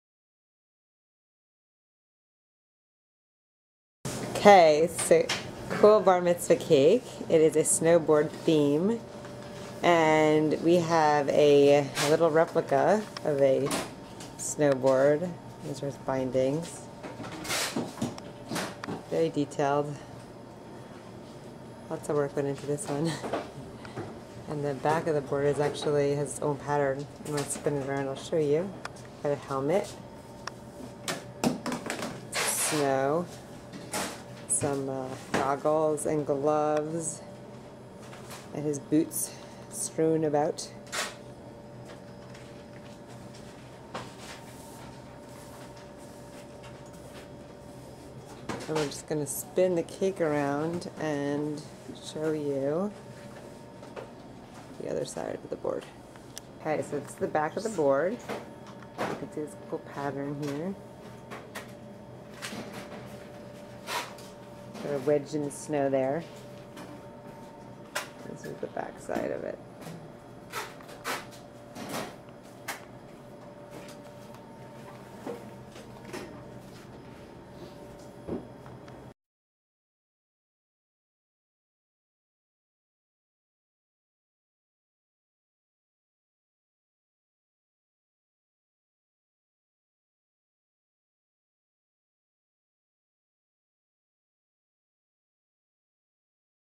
Bar Mitzvah Snowboard Cake
Right in time for the upcoming snowboarding season, here's a four tier topsy turvy cake complete with a snowboard, helmet, boots, gloves and goggles.